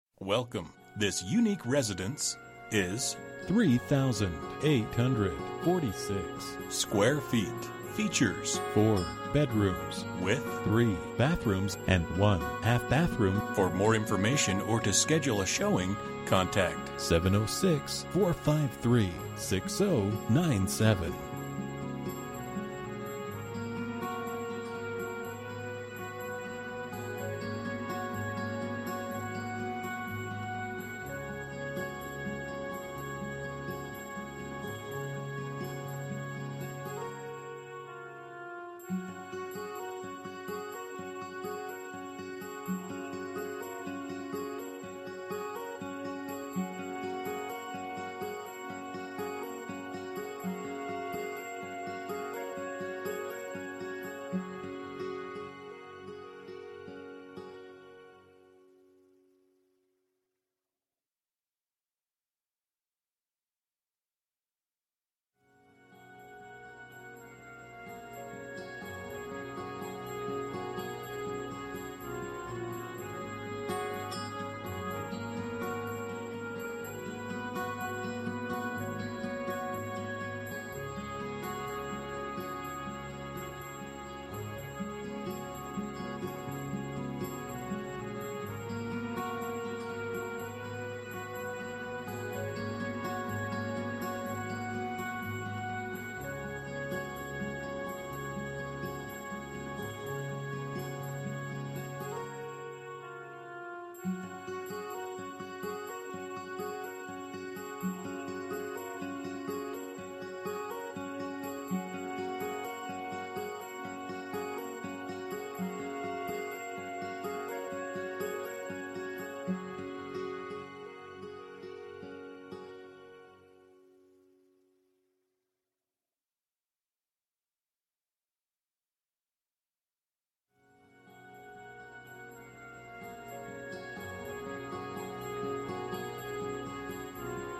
Residential for sale - 1591 ARROWHEAD ROAD, Greensboro, GA 30642
a wonderful cape cod on Lake Oconee.  All the space you could ever need with over 3846 Sqft. Two Storys with a full walk out basement, 4 Bd/ 3.5 BAs, 2 kitchens, two great rooms, sun room, office. workshop, 2 car carport, detached boat storage. Dock, granite seawall, 1.2 Acre lot, with huge views of beautiful Lake Oconee. Easy access to Athens, Ga. only 30 miles, and 70 miles to Atlanta.Bedrooms: 4Bathrooms: 3 full and 1 half BathroomsSquare Feet: 3846Price: $275,000MLS ID: 45580For more information about this property, please contact LAMAR CALLAWAY at or .
11/14/2016 12:50:01 amLast modified: